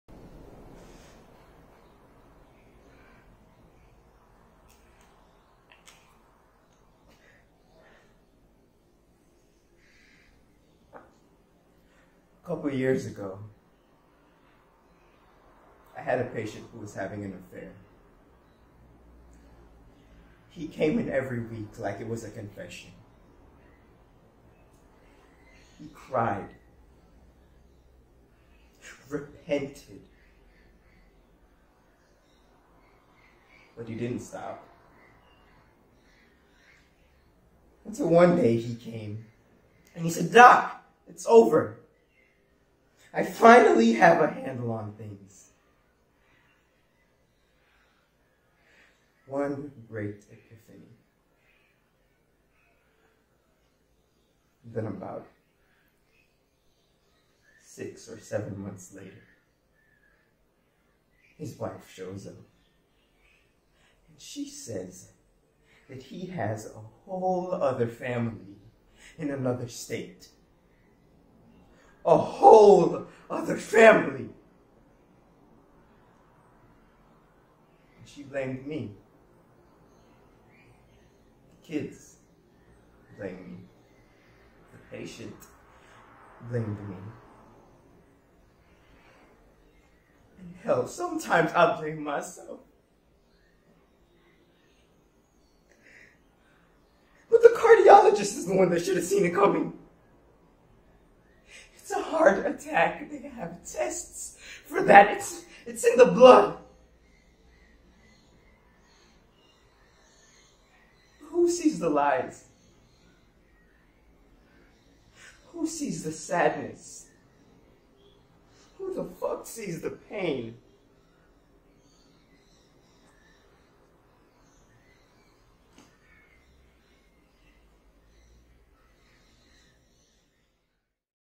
DR SIEBERT - SULE THELWELL
Steven Soderbergh’s film “SIDE EFFECTS”

Hello, my name is Sule Kamu Thelwell and I am a 19 year old Jamaican actor who got into one of the top 5 Drama schools in the world, but I'm not able to finance my education. If you have the time please hear my story.